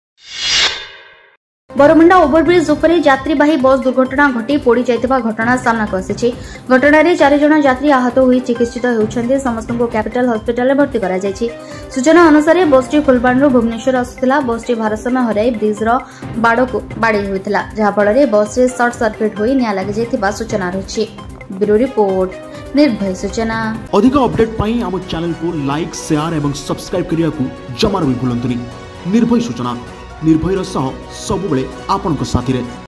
ସର୍ଟ ସର୍କିଟ ଯୋଗୁଁ ନିଆଁ ଲାଗିଥିବା ସନ୍ଦେହ
ବରମୁଣ୍ଡା ଓଭର ବ୍ରିଜ ଉପରେ ଯାତ୍ରୀବାହୀ ବସ ଦୁର୍ଘଟଣା ଘଟି ପୋଡି ଯିବା ଘଟଣା । ଘଟଣାରେ ୪ ଜଣ ଯାତ୍ରୀ ଆହତ ହୋଇ ଚିକିତ୍ସା ହେଉଛନ୍ତି